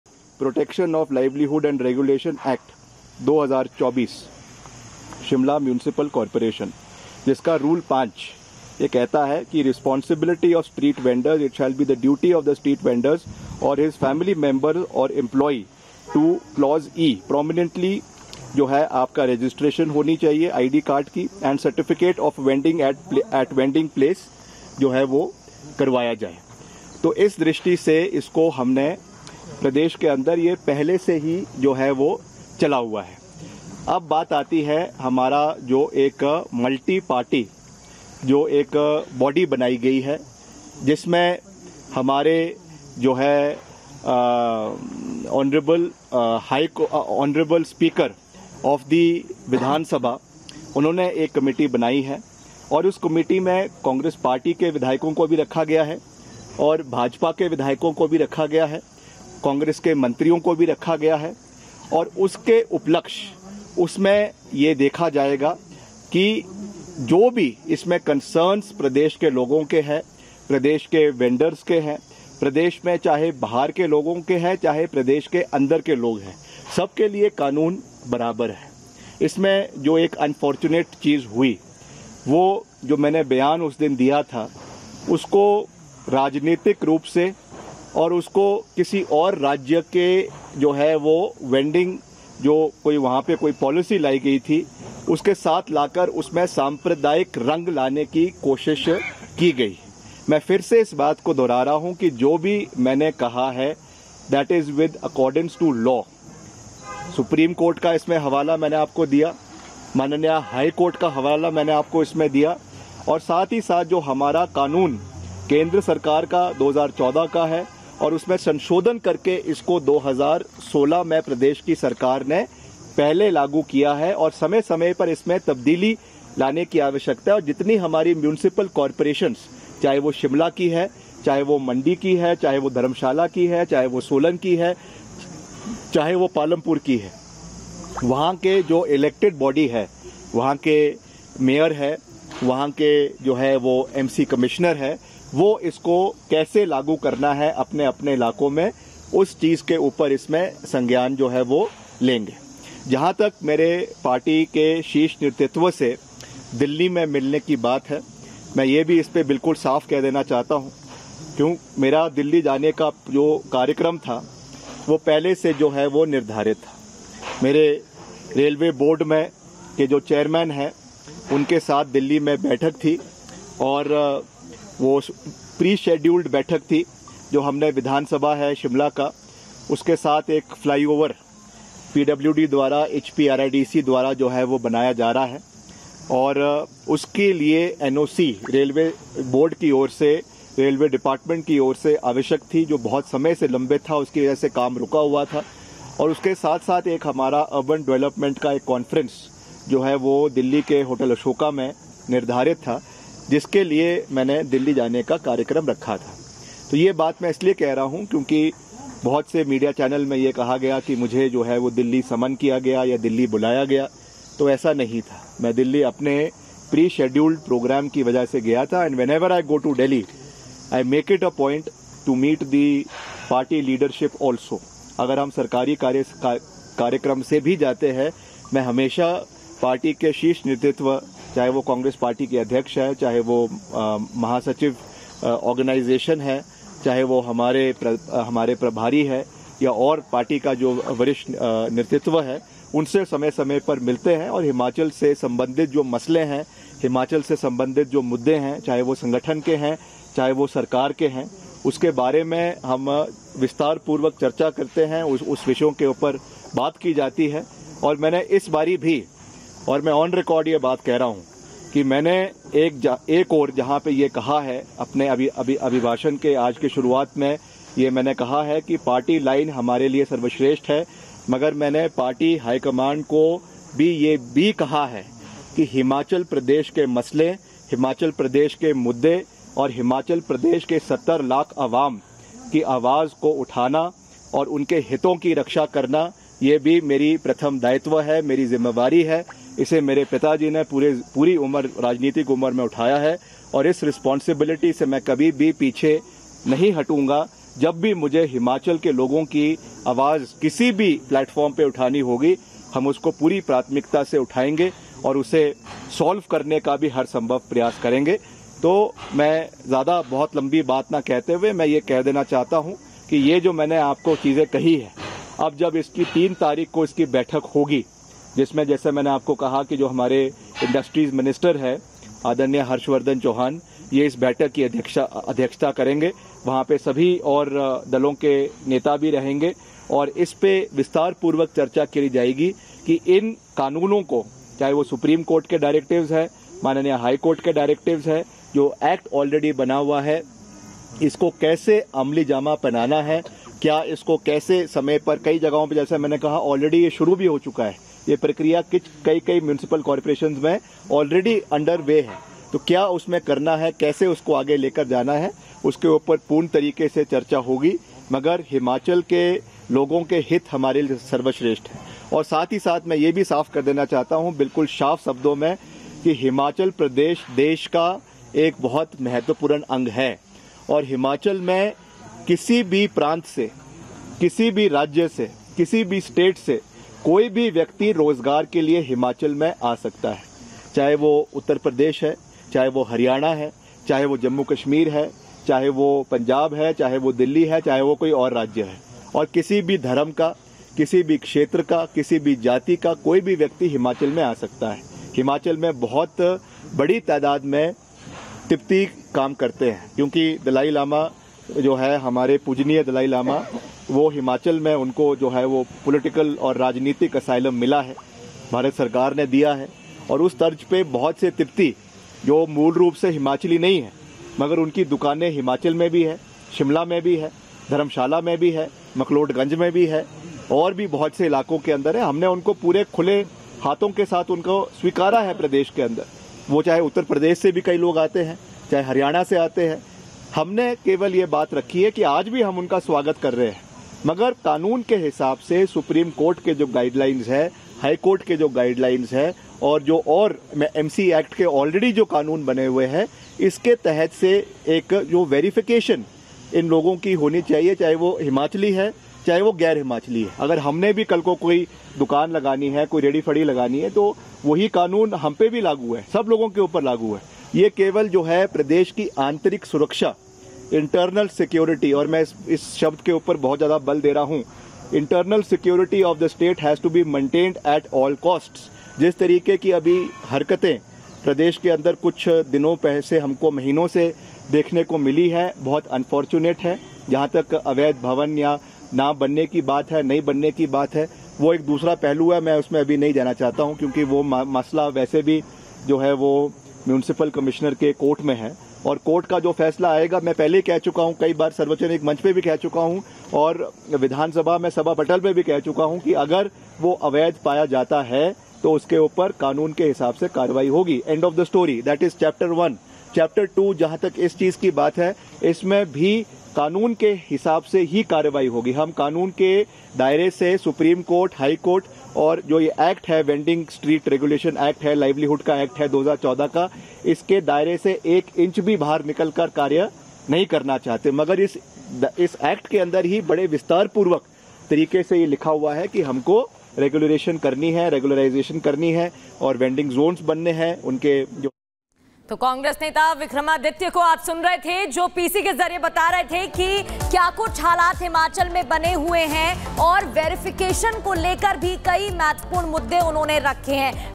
"...हमारी मंशा बिल्कुल साफ थी कि हमें हिमाचल के हितों की - Vikram Aditya | R Bharat
Himachal News: विक्रमादित्य सिंह ने कहा कि प्रदेश की जनता की आवाज उठाना हमारी प्राथमिकता है. उन्होंने साथ ही कहा कि हिमाचल में किसी भी प्रांत के लोग आकर रोजगार कर सकते हैं.